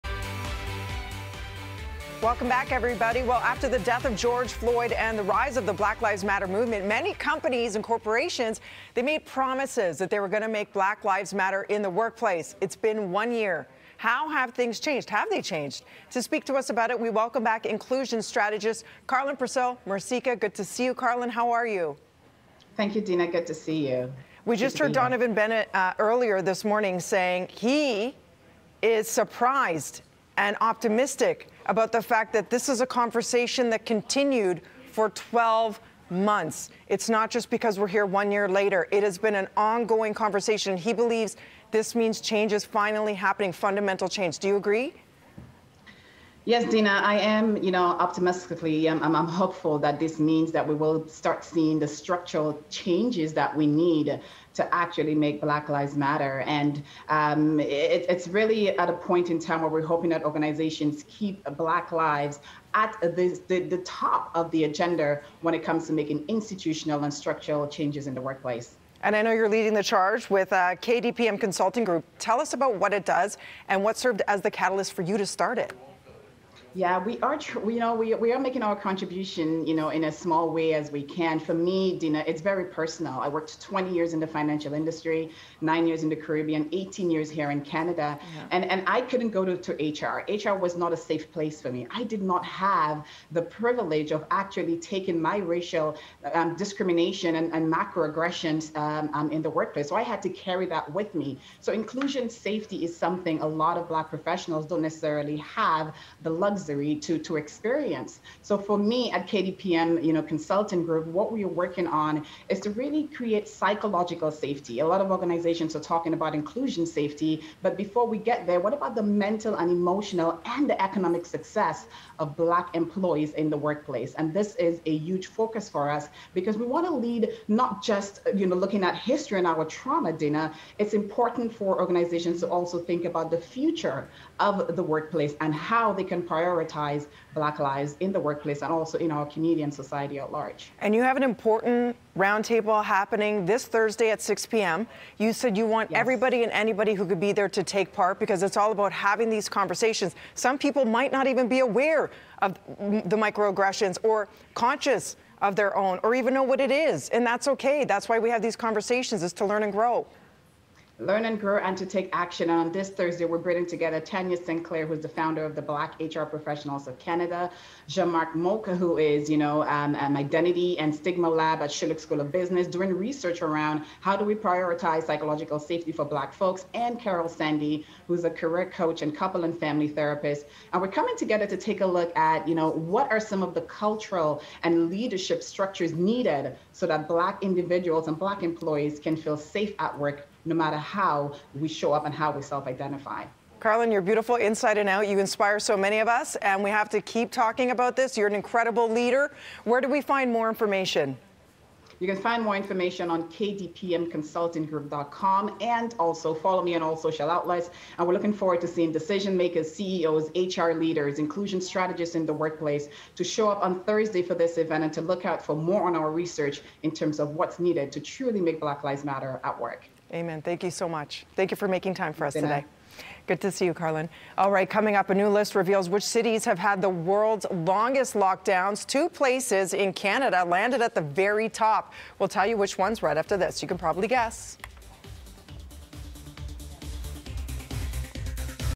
BLM in the Workplace
Today marks a year since the murder of George Floyd. Dina is joined by Inclusion Strategist, Karlyn Percil-Mercieca, to learn about what companies can and should do to continue fostering an inclusive environment for their employees.